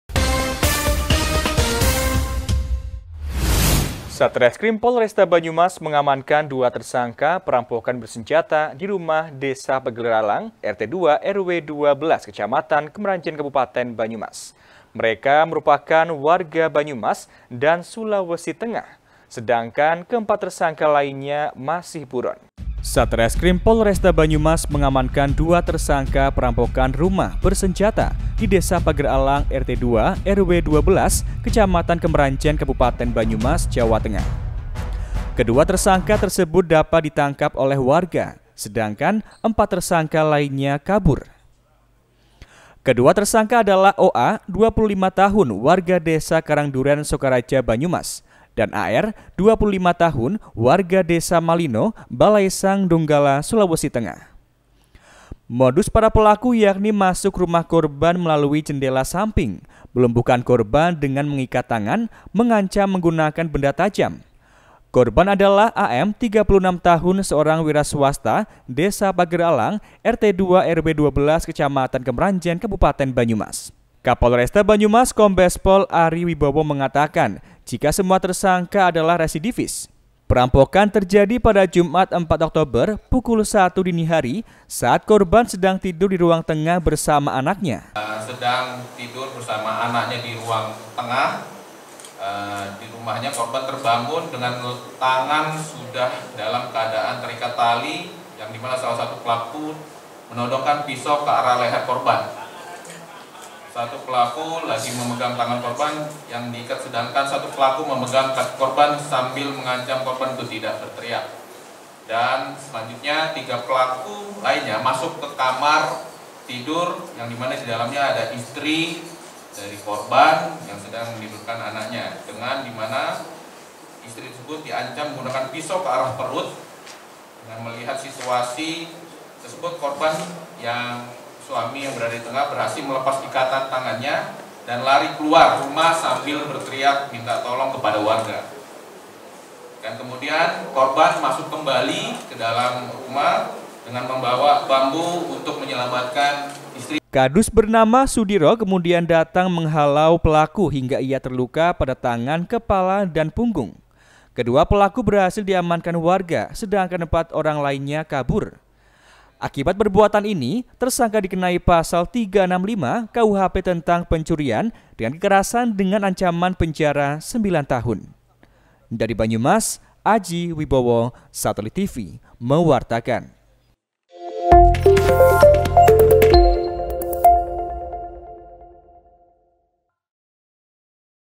DUA PERAMPOK BERSENJATA DI PAGERALANG KEMRANJEN BANYUMAS DITANGKAP, EMPAT MASIH BURON
Satreskrim polresta Banyumas mengamankan 2 tersangka perampokàn bersenjatà di rumah desa Pageralang,  RT 2 / RW 12, Kecamatan Kemranjen, Kabupaten Banyumas. Mereka merupakan warga Banyumas dan Sulawesi Tengah.  Sedangkan ke 4 tersangka lainnya masih buron.